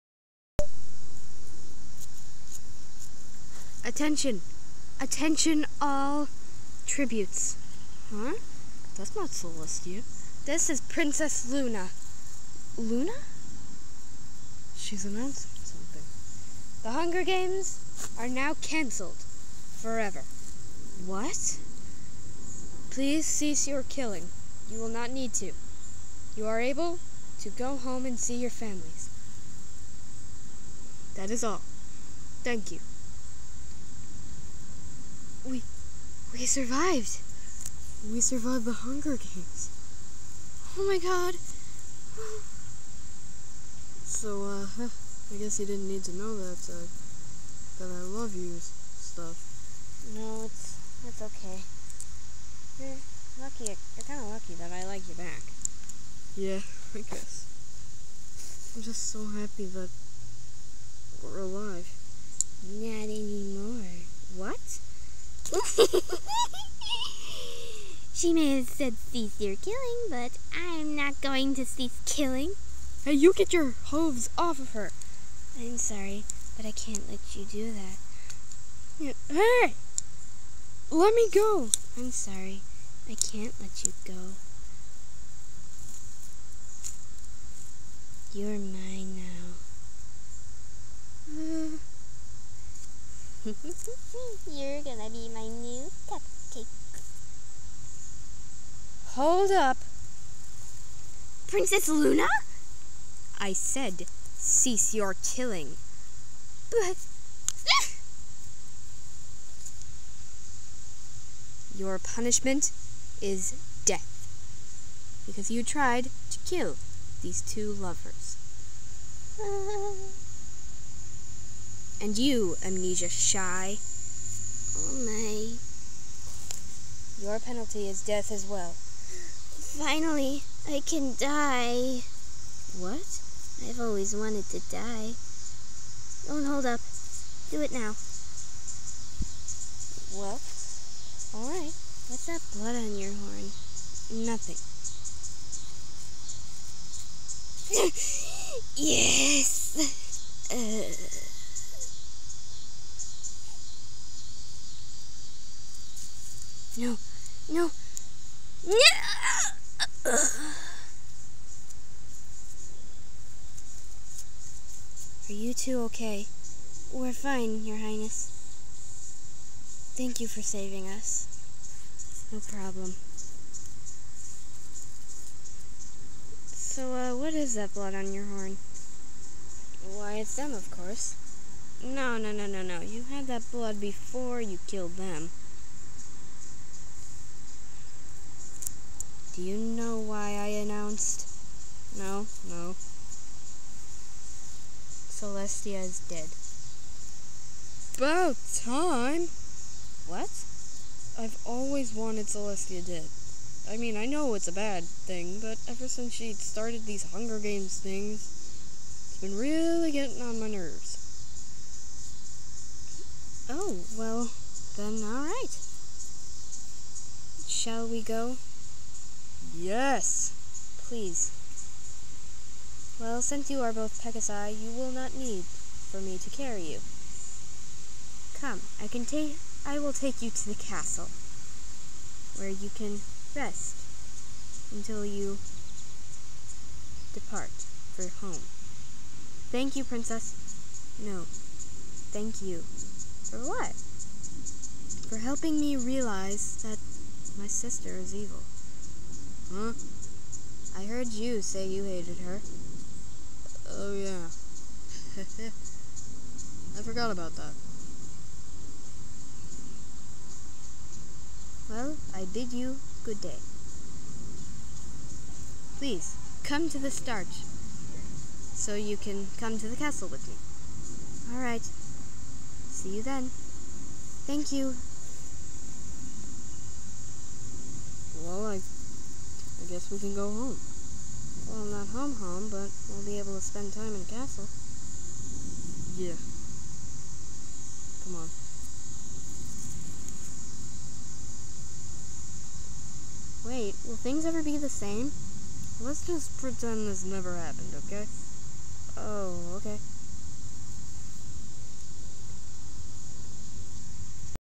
Mlp:Hunger Games Season 3 ep.2 (Canceled Forever)
Princess Luna makes an announcement that the Hunger Games are canceled forever. But that doesn't stop Pinkamena and Amnesiashy.